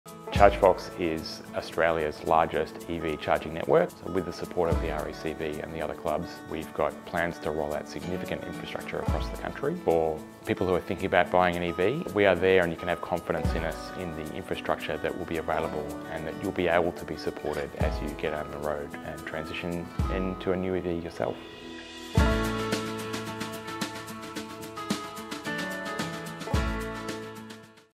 RACV and Chargefox are growing Australia’s network of EV charging stations
RACV has invested in Chargefox to help its customers transition to a cleaner energy future. As more Australian car owners make the transition to electric vehicles, they can feel confident that the infrastructure is there to support them on daily commutes and weekend road trips with more EV charging stations being rolled out across the country.